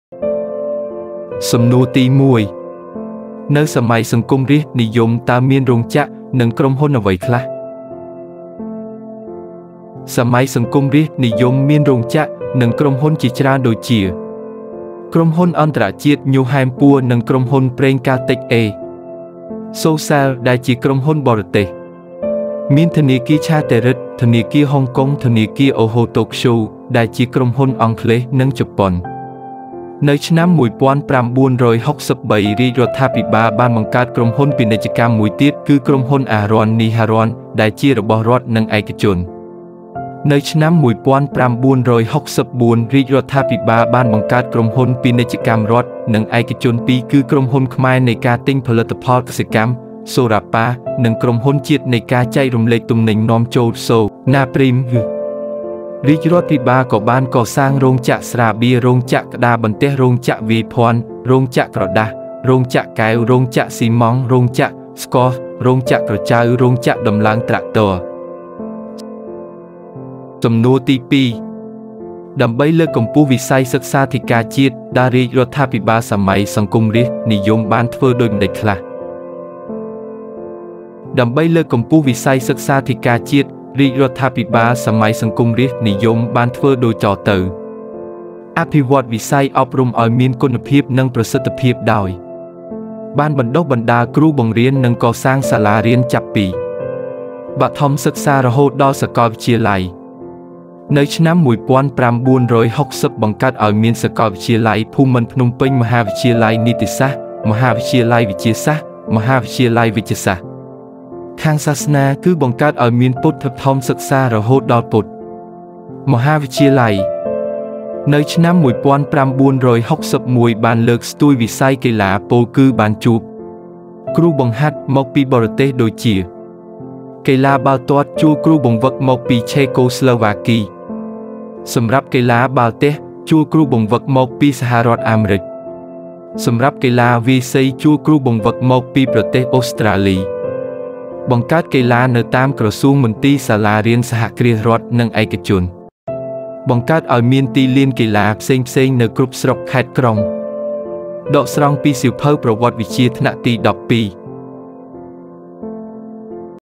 ដើម្បីលើកកម្ពស់វិស័យសិក្សាធិការជាតិតើរាជរដ្ឋាភិបាលសម័យ សង្គមរាស្ត្រនិយមបានធ្វើដូចម្តេចខ្លះ| My QandA
សំណួរទី១

នៅសម័យសង្គមរាស្ត្រនិយម តើមានរោងចក្រ និងក្រុមហ៊ុនអ្វី
ខ្លះ?
សម័យសង្គមរាស្ត្រនិយម មានរោងចក្រ និងក្រុមហ៊ុនជាច្រើនដូចជា៖

ក្រុមហ៊ុនអន្តរជាតិ ញូហែមពួរ និងក្រុមហ៊ុន ប្រេងកាល់តិច អេ
សូ សែល ...ដែលជាក្រុមហ៊ុនបរទេស

មានធនាគារឆារទែរិដ ធនាគារហុងកុង ធនាគារអូហូតូក្យូ ដែលជាក្រុមហ៊ុនអង់គ្លេស និងជប៉ុន

នៅឆ្នាំ១៩៦៣ រាជរដ្ឋាភិបាលបានបង្កើតក្រុមហ៊ុនពាណិជ្ជកម្មមួយទៀតគឺក្រុមហ៊ុនអាហរ័ណ នីហរ័ណ  ដែលជារបស់រដ្ឋ និងឯកជន

នៅឆ្នាំ ១៩៦៤ រាជរដ្ឋាភិបាលបានបង្កើតក្រុមហ៊ុន ពាណិជ្ជកម្មរដ្ឋ និងឯកជនពីរគឺក្រុមហ៊ុនខ្មែរនៃការទិញផលិតផលកសិកម្ម (សូរ៉ាប៉ា ) និងក្រុមហ៊ុនជាតិនៃការចែករំលែកទំនិញនាំចូល (សូ
ណាព្រីម)

រាជរដ្ឋភិបាលក៏បានកសាងរោងចក្រស្រាបៀ រោងចក្រក្តារបន្ទះរោងចក្រវាយភ័ណ្ឌ រោងចក្រក្រដាស រោងចក្រកែវ រោងចក្រស៊ីម៉ង់ រោងចក្រ ស្ករស រោងចក្រក្រចៅ រោងចក្រដំឡើងត្រាក់ទ័រ។

សំណួរទី២

ដើម្បីលើកកម្ពស់វិស័យសិក្សាធិការជាតិ តើរាជរដ្ឋាភិបាលសម័យ សង្គមរាស្ត្រនិយមបានធ្វើដូចម្តេចខ្លះ?

ដើម្បីលើកកម្ពស់វិស័យសិក្សាធិការជាតិ រាជរដ្ឋាភិបាល សម័យសង្គមរាស្ត្រនិយម បានធ្វើដូចតទៅ ៖

អភិវឌ្ឍ វិស័យ អប់រំឲ្យមានគុណភាពនិងប្រសិទ្ធភាព ដោយ
បានបណ្ដុះបណ្តាលគ្រូ បង្រៀន និងកសាងសាលារៀន ចាប់ពី
បឋមសិក្សារហូតដល់សាកលវិទ្យាល័យ។

នៅឆ្នាំ ១៩៦០ បង្កើតឲ្យមានសាកលវិទ្យាល័យ ភូមិន្ទភ្នំពេញមហាវិទ្យាល័យនីតិសាស្ត្រ មហាវិទ្យាល័យវិទ្យាសាស្ត្រ មហាវិទ្យាល័យវេជ្ជសាស្ត្រ ...។

ខាងសាសនា គឺ បង្កើតឲ្យមានពុទ្ធិបឋមសិក្សា រហូតដល់ពុទ្ធិ
មហាវិទ្យាល័យ ។

នៅឆ្នាំ ១៩៦១ បានលើក ស្ទួយវិស័យ កីឡា ពោលគឺ បានជួប
គ្រូបង្ហាត់ មកពីបរទេសដូចជា ៖

កីឡាបាល់ទាត់ ជួលគ្រូបង្វឹកមកពី ឆេកូស្លូវ៉ាគី ។

សម្រាប់កីឡា បាល់ទះ ជួលគ្រូបង្វឹកមក ពីសហរដ្ឋអាមរិក ។

សម្រាប់កីឡា វាយសី ជួលគ្រូបង្វឹកមក ពី ប្រទេសអូស្ត្រាលី ។

បង្កើតកីឡានៅតាម ក្រសួង មន្ទីរ សាលា រៀន សហគ្រាសរដ្ឋ និងឯកជន ។

បង្កើតឲ្យមានទីលានកីឡា ផ្សេងផ្សេង នៅគ្រប់ស្រុក ខេត្ត ក្រុង ។